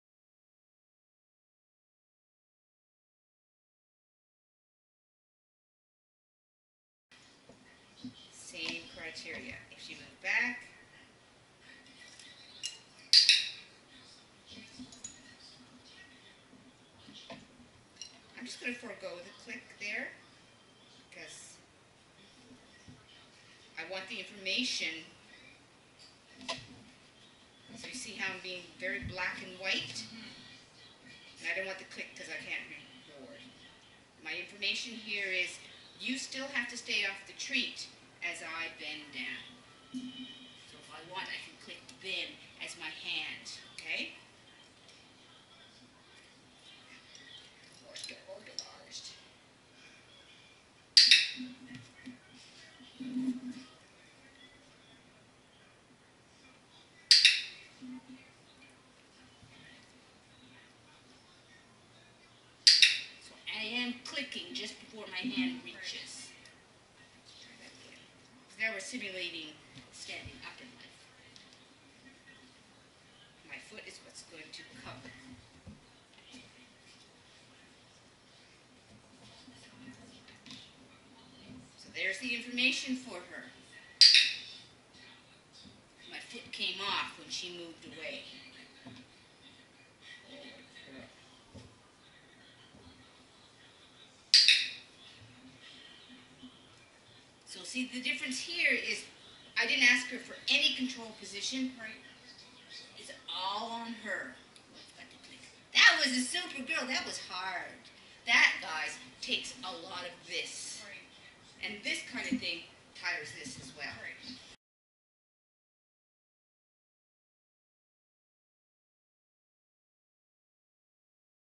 Rosie iyc owner standing
Teaching impulse control -adding new criteria -owner standing